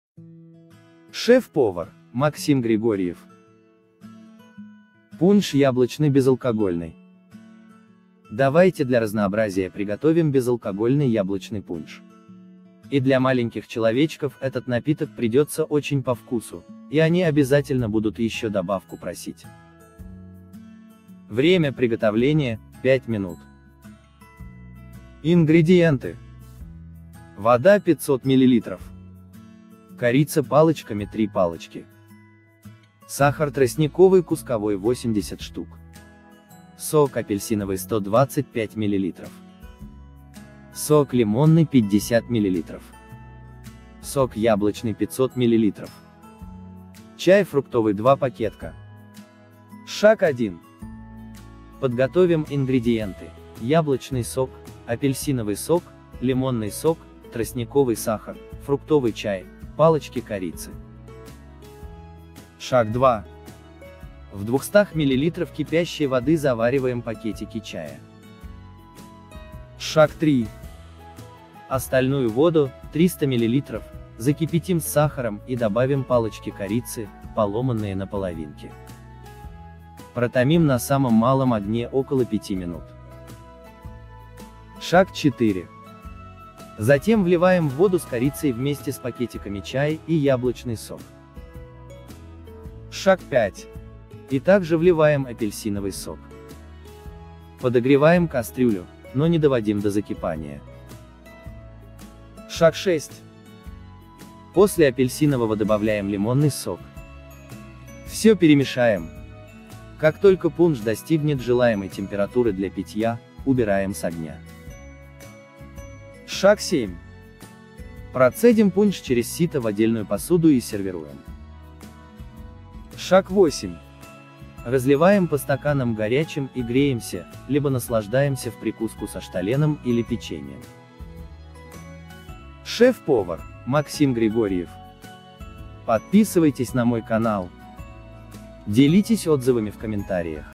Пунш яблочный безалкогольный . Рецепт от шеф повара Максима Григорьева
Давайте для разнообразия приготовим безалкогольный яблочный пунш. И для маленьких человечков этот напиток придётся очень по вкусу, и они обязательно будут ещё добавку просить.